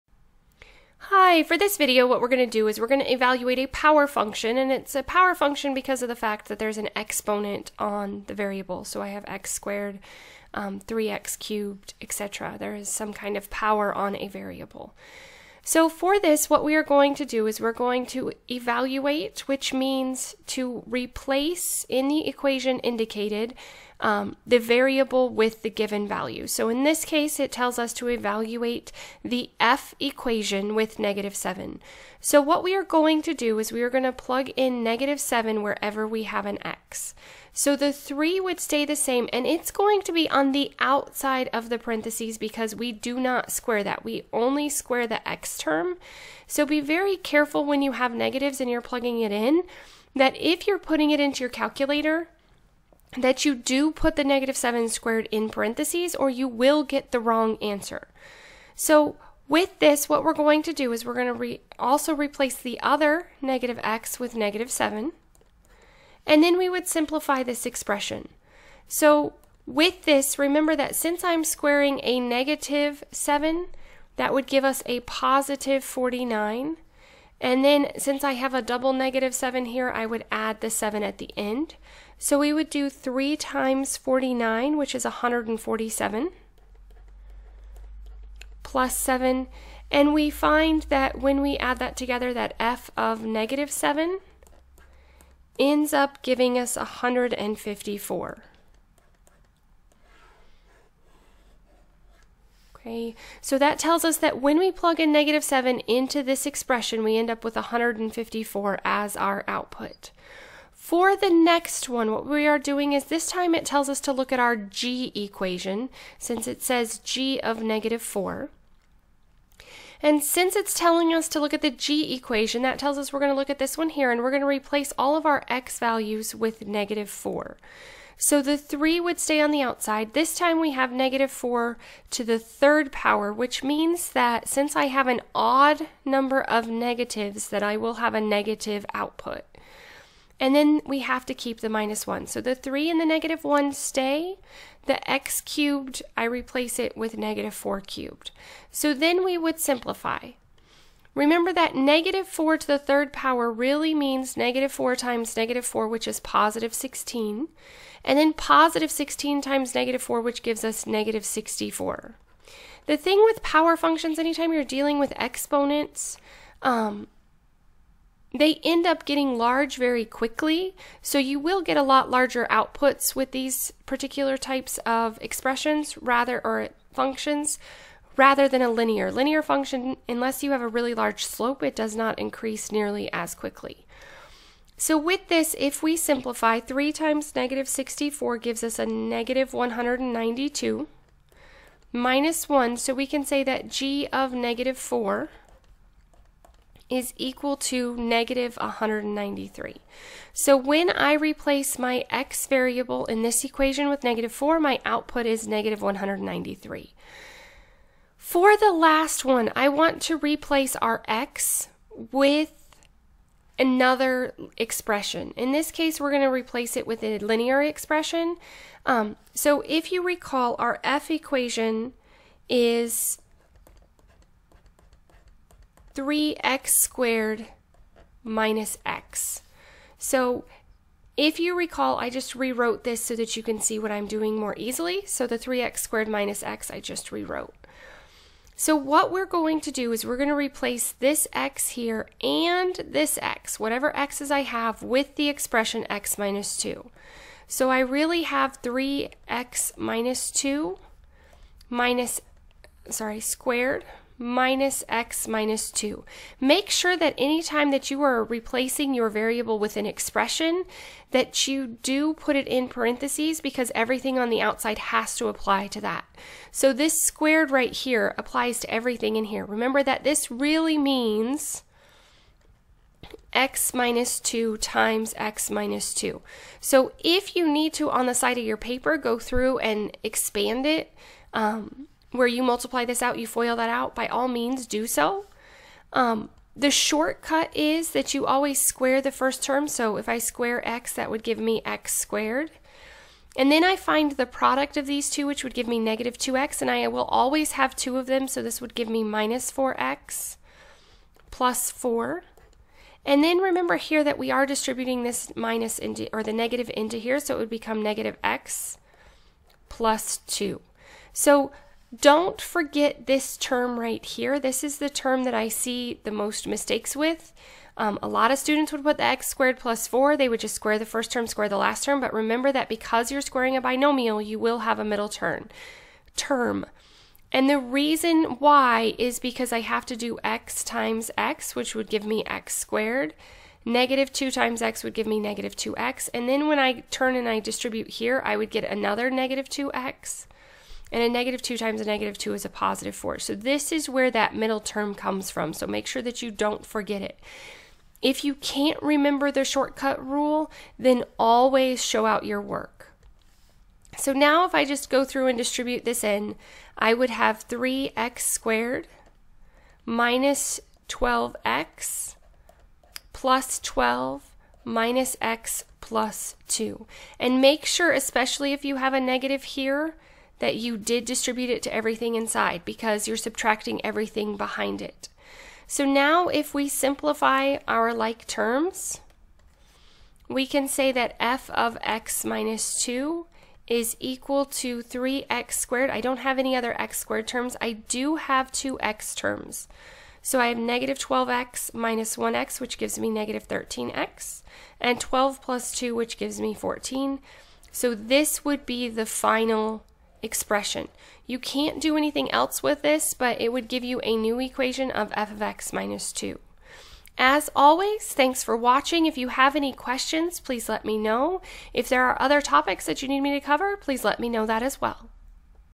Evaluate a Power Function (a function with exponents on the variables)
Learn how to evaluate a function that has exponents on the variables.